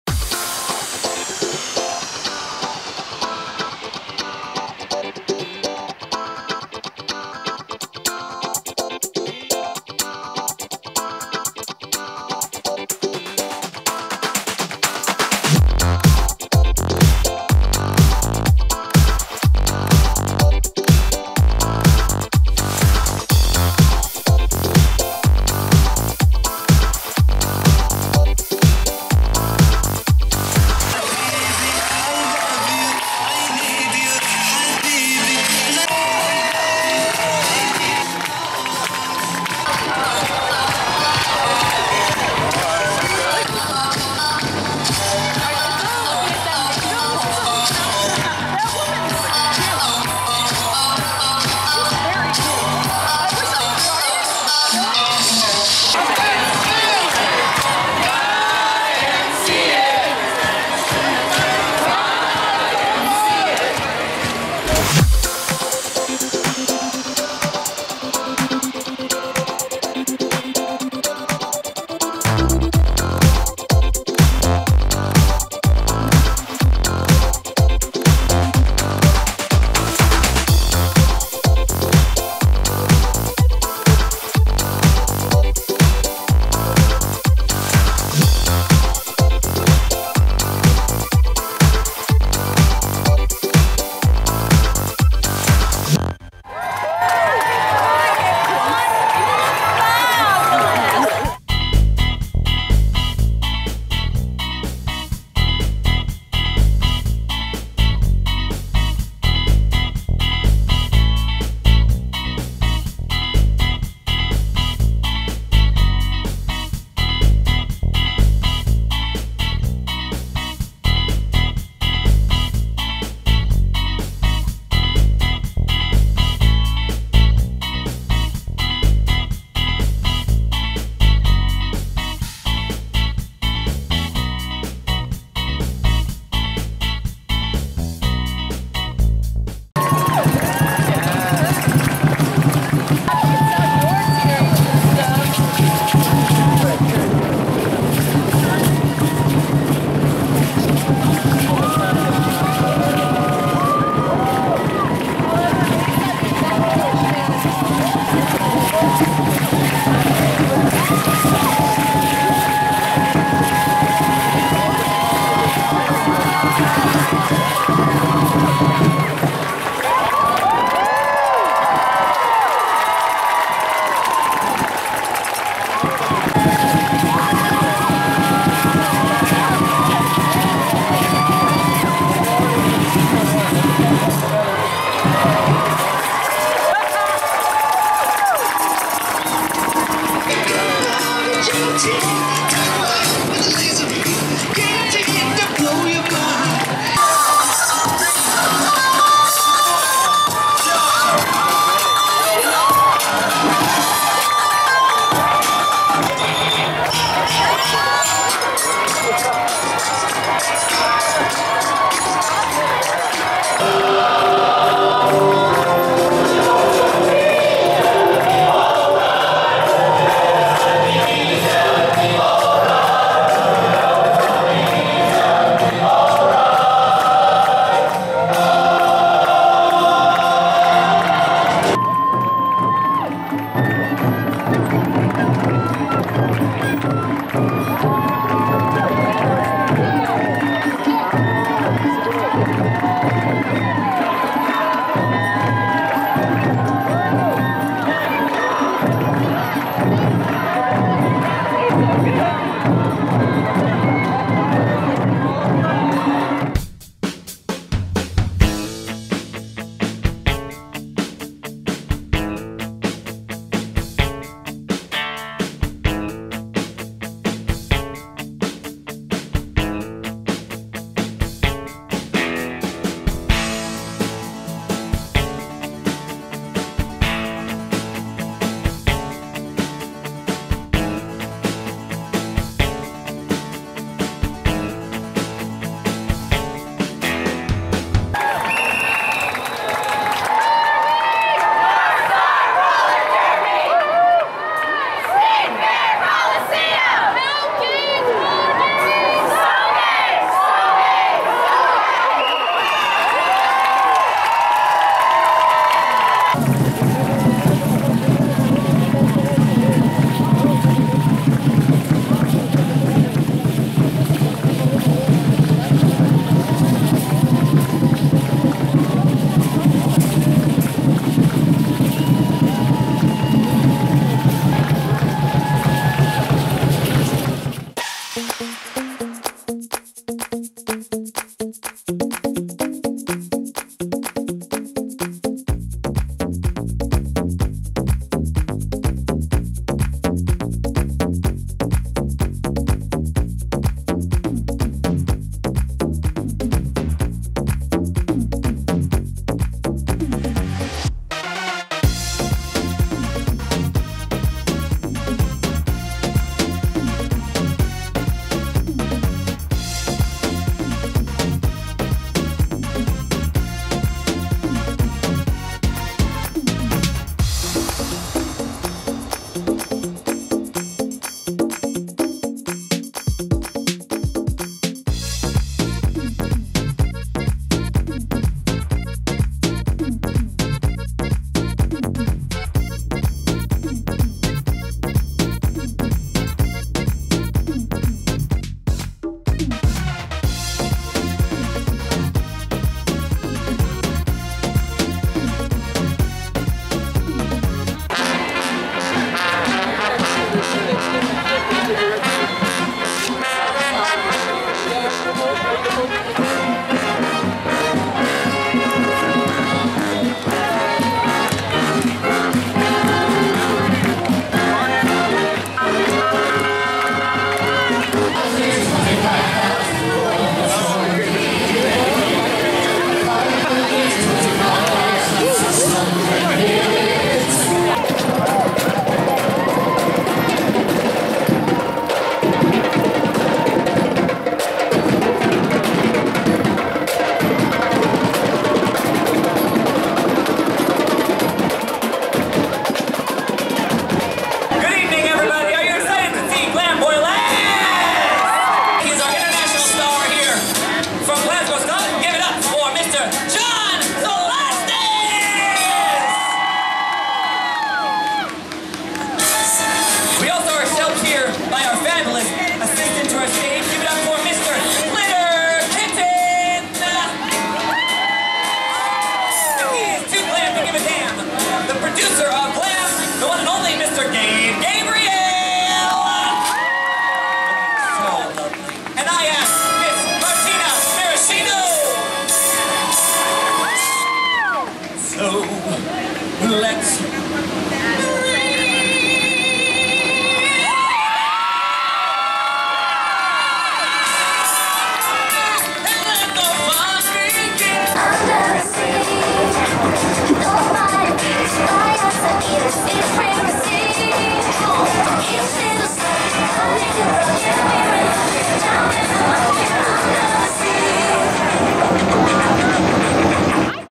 twin cities pride 2017
a parade, boylesque, drag, what more could a gay want?

kaihugstrees on snapchat

hi! I'm Kai and I like to hug trees.